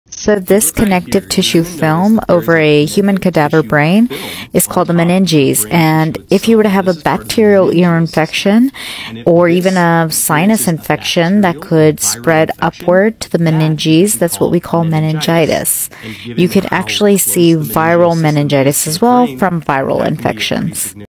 What is Meningitis? Pediatrician Explains! | Dr. Amna Husain
Follow along as board-certified pediatrician and lactation consultant Dr. Amna Husain explains what meningitis is in this video

***The information in this video is intended to serve as educational information and can not be construed as a substitute for professional medical advice, diagnosis, or treatment of you or your child.